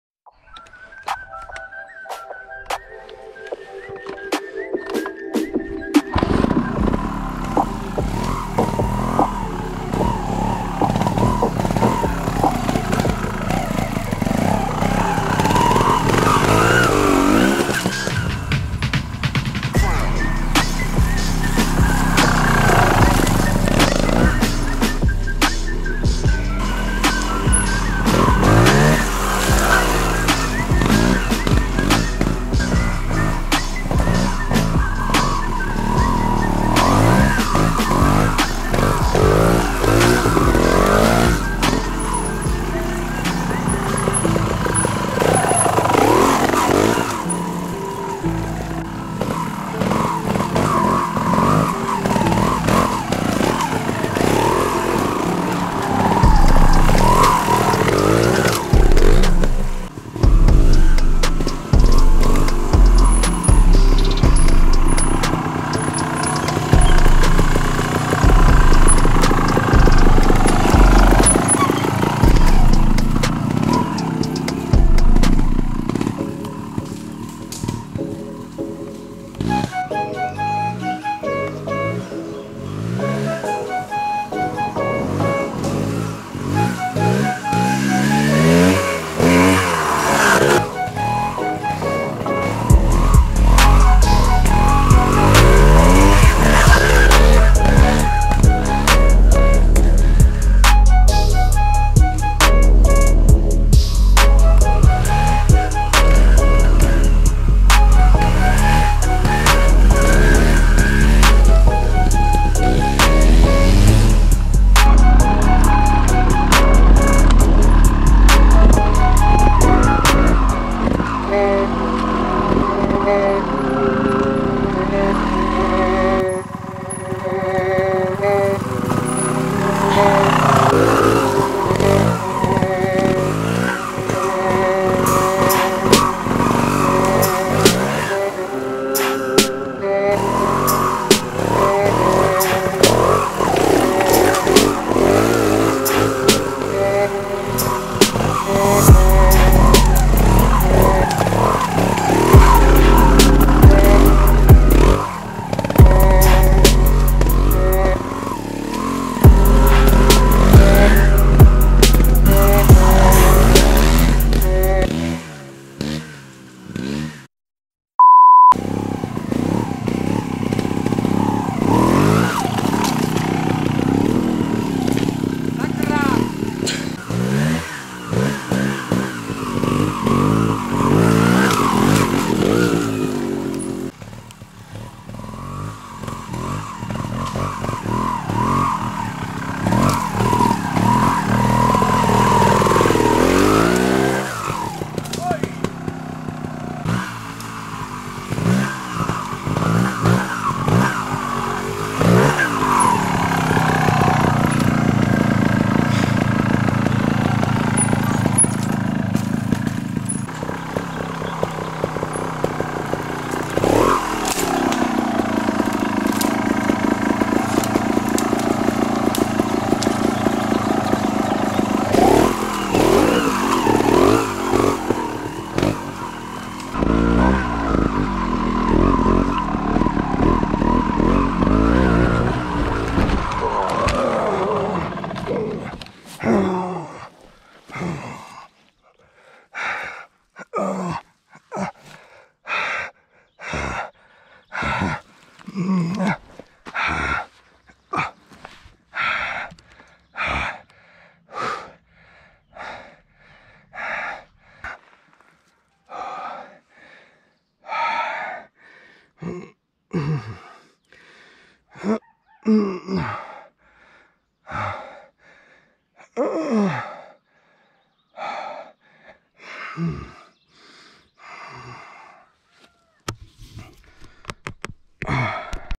How I Almost Got Myself Killed On Trials Bike! Scorpa Free Riding
Stupid me, my mistake with a little bit of stuck throttle.